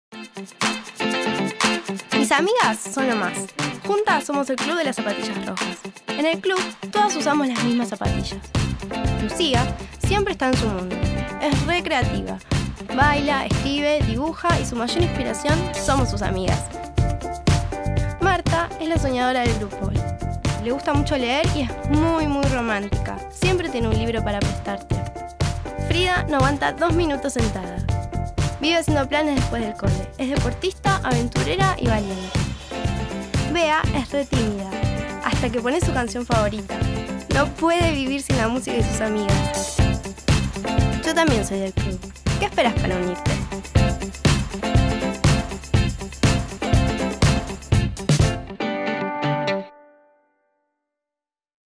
El club de las zapatillas rojas Booktrailer
Lucía, Frida, Bea y Marta deciden sellar un pacto de amistad y fundan ElClubDeLasZapatillasRojas Conocé a las chicas del Club y animate vos también a formar parte.